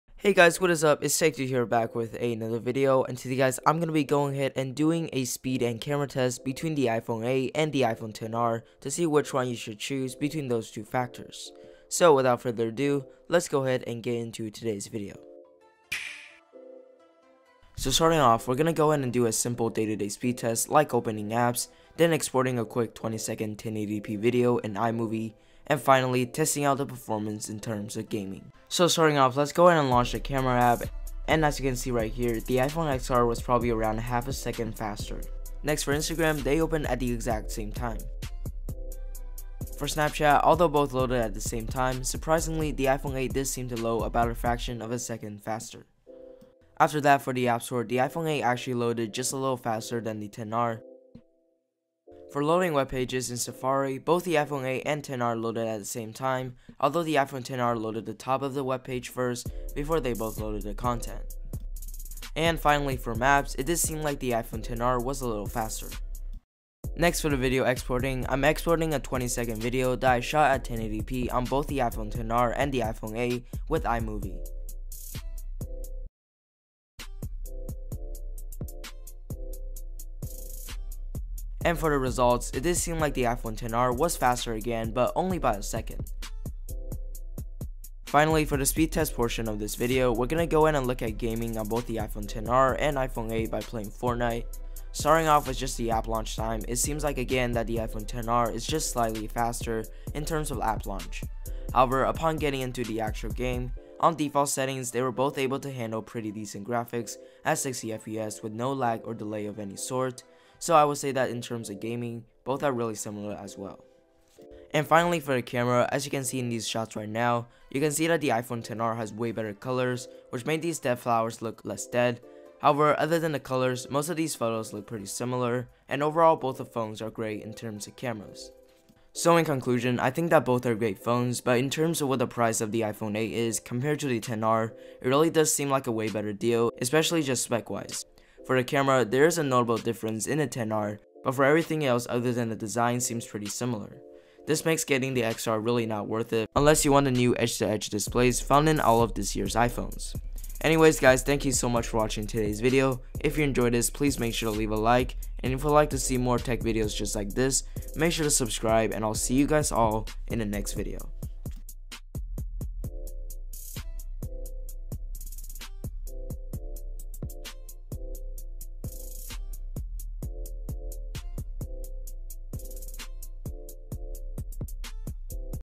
iPhone 8/8 Plus vs iPhone XR in 2019(Speed test)(Camera test)
Tech Dude does speed/performance/Camera test between the apple iPhone 8/8 plus and iPhone XR in 2019